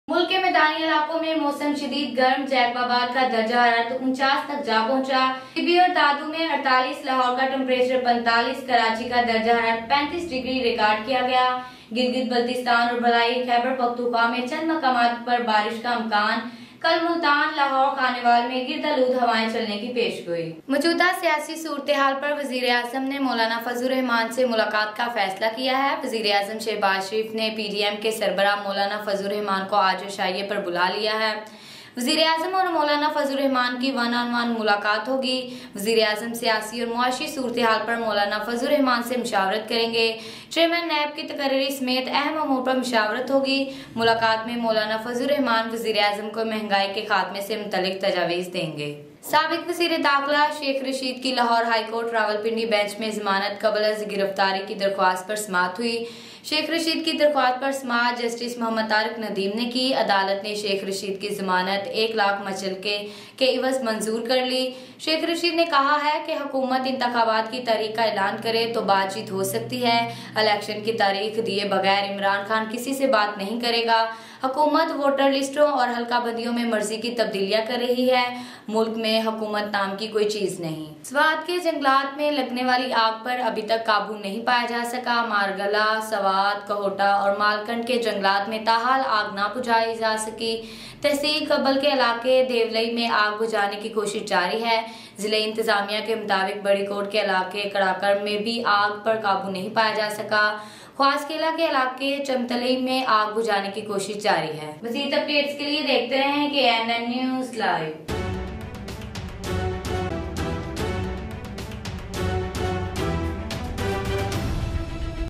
SAWAT K Janglat Mai Lagi Aag Ab Tak Beqbo # KNN NEWS LIVE 6 June 2022
SAWAT K Janglat Mai Lagi Aag Ab Tak Beqbo
Sheikh Rasheed Ki Zamanat Manzor Kar Li Gayi
PM Shahbaz Sharif Ka Molana Fazal Ul Rehman  Se Mulaqaat Ka Faisla
Mulk K Medani Elaqo Mai Mosam Shadid Garm